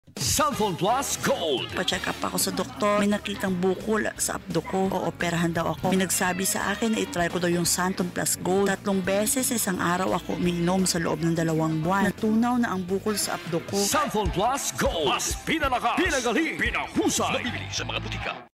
Xanthone Plus Gold Radio Commercial 2021 18s (Testimonial, Version 4)
This Radio Commercial was sourced from DZBB-AM 594 KHz Super Radyo DZBB Manila.

DRPS Two Episode No. 730: Xanthone Plus Gold Radio Commercial 2021 18s (Testimonial, Version 4)